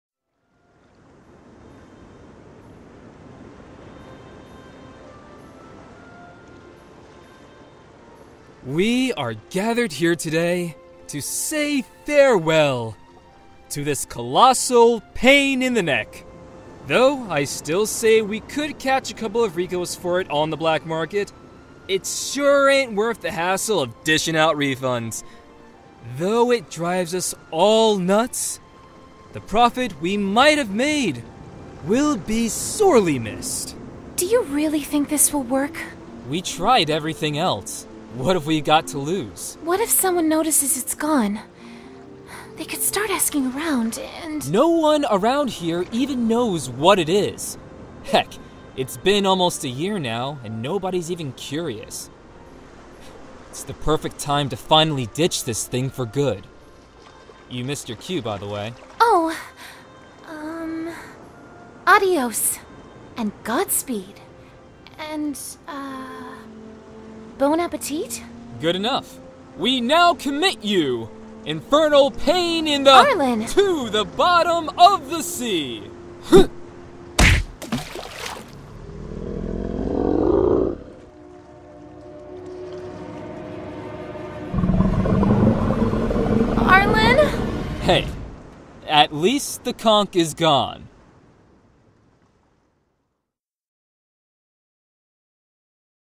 [Scene 1] High-Seas Honey (Progress Report)
VOICE ACTORS:
RamuneAmai as Arlan
HeartofAnxiety as Cordelia

Hello everyone! I'm so sorry for being AWOL for a million years. Whew, it's been forever since my desktop computer keeled over and took all my files along with it. I couldn't afford to have it fixed while I've been at university because tuition's been the priority, but I'll go ahead and share with you a scene I had done back before all the tech issues killed me for like, a year or two. Sheesh!

Looking at it now, I know it still needs some work and polish. That conch toss Arlan does is waaayyy too slow (MATRIX CONCH!?), and he and Cordelia don't move nearly as much as I'd like (the somewhat stiff style is meant to save time 'cause this is pretty much a one-woman animating team here and I'm still sort of learning), and I really think they should've been looking down at the ocean before the serpent rose up out of it.

Gosh, LOTS of work needs to be done to improve this thing. xD But I know you guys have been waiting and amazingly some of you are still commenting and leaving me messages. O__O Definitely didn't expect that. So if nothing else, I'll upload the progress I DID make here on the channel. If anyone's been following the High-Seas Honey Tumblr (highseashoney.tumblr.com) then you'd have seen at least some of these clips already, but here goes!

-- Kn